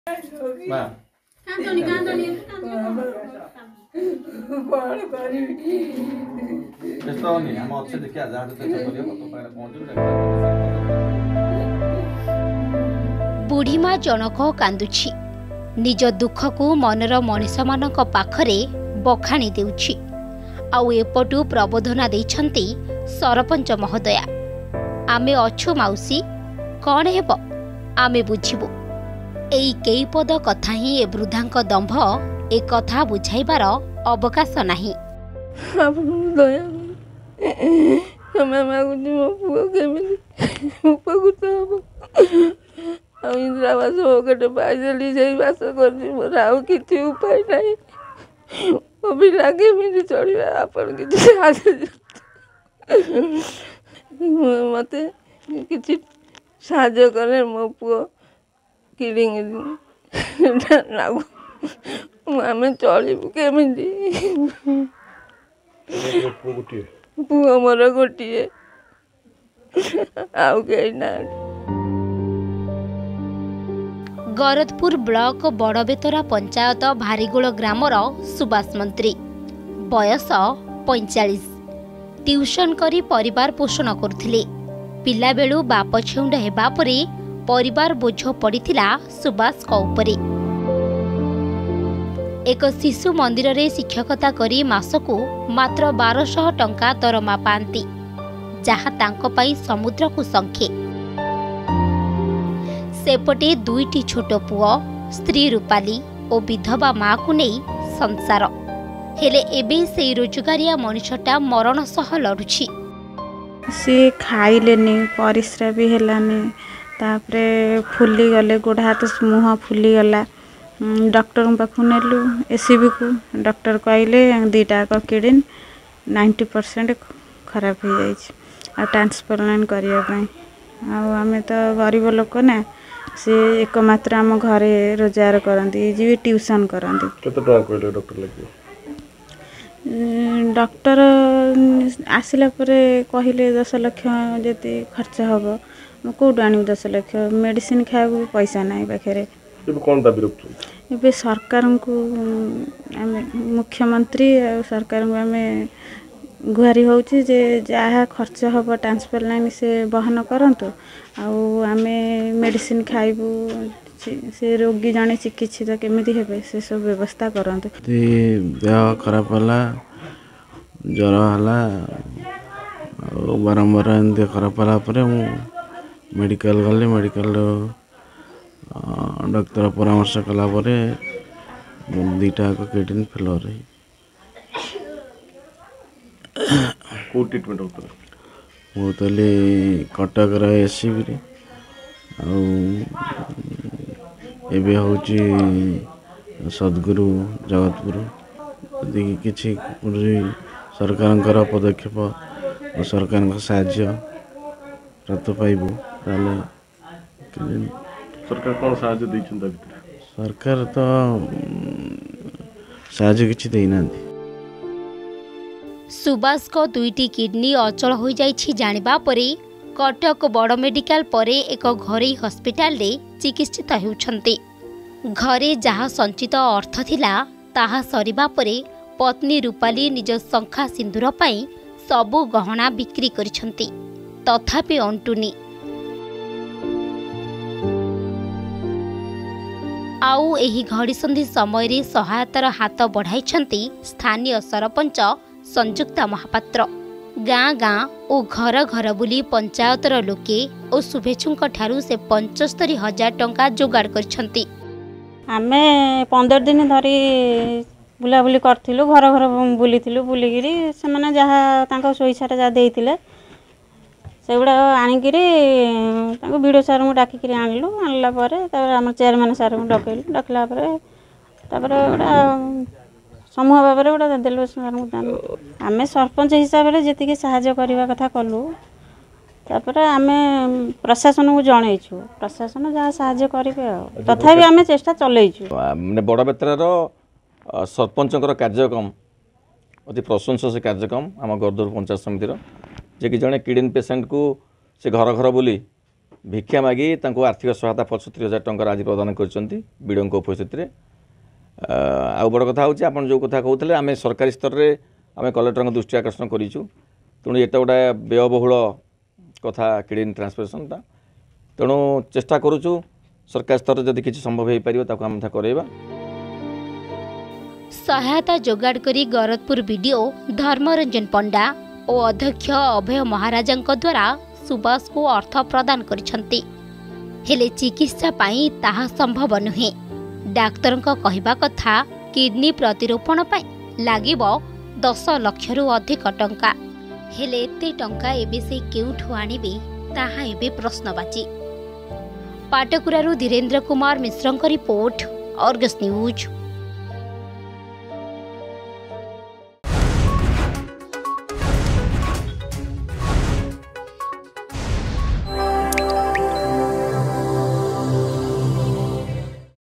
ଗରଦପୁର ବ୍ଲକ ବଡ଼ଵେତରା ପଞ୍ଚାୟତ ଭାରିଗୋଳ ଗ୍ରାମର ସୁବାସ ମନ୍ତ୍ରୀ
ଟିଉସନ କରି ପରିବାର ପୋଷଣ କରୁଥିଲେ । ପିଲାବେଳୁ ବାପା ଛେଉଣ୍ଡ ହେବା ପରେ ପରିବାର ବୋଝ ପଡ଼ିଥିଲା ସୁବାସଙ୍କ ଉପରେ । ହେଲେ ଏବେ ସେ ପରିବାର ପାଇଁ ବୋଝ। ରୋଗବାଧିକି ତାଙ୍କ ଅଣ୍ଟା ଭାଙ୍ଗି ଦେଇଛି। ହେଲେ  ଏତିକି ବେଳେ ସହାୟତାର ହାତ ବଢାଇଛନ୍ତି ଖୋଦ୍ ସରପଞ୍ଚ।